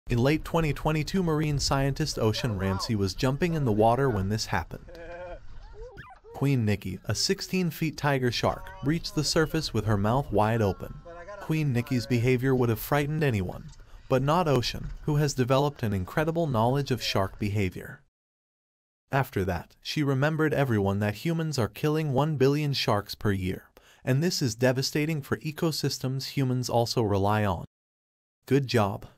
What if this happens to you, while you're jumping in the water?
In this riveting video from November 2022, renowned marine scientist Ocean Ramsey has an unexpected encounter just before a dive. Queen Nikki, a majestic 16-foot tiger shark, breaches the surface, displaying her immense size and power. While this sight would instill fear in most people, Ocean's deep understanding of shark behavior allows her to appreciate this extraordinary moment.

Ocean uses this experience to remind us of the crucial role that sharks play in our ecosystem. We are currently killing an estimated 1 billion sharks per year, with devastating consequences for the environment we depend on.

Join us in exploring the wonders of the underwater world and promoting its conservation.